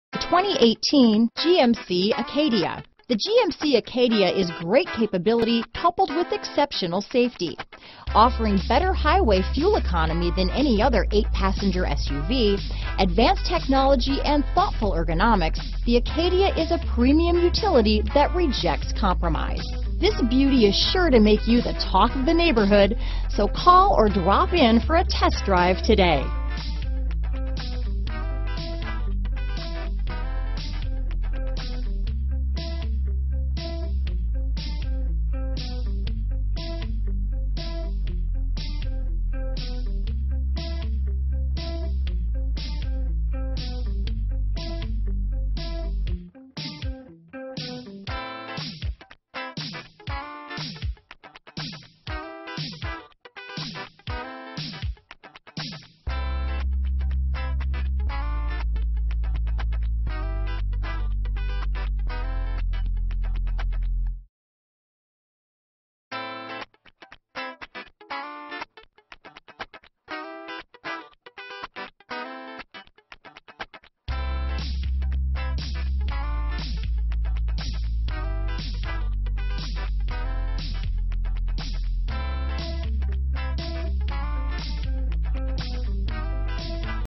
2018 GMC Acadia Gurnee, Waukegan, Kenosha, Arlington Heights, Libertyville, IL G1212
Iridium Metallic [LISTING TYPE] 2018 GMC Acadia available in Gurnee, Illinois at Anthony Buick. Servicing the Waukegan, Kenosha, Arlington Heights, Libertyville, IL area.